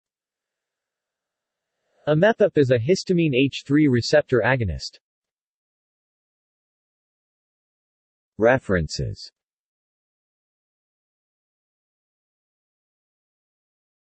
Immepip | Wikipedia audio article
This is an audio version of the Wikipedia Article:
Immepip

SUMMARY
Immepip is a histamine H3 receptor agonist.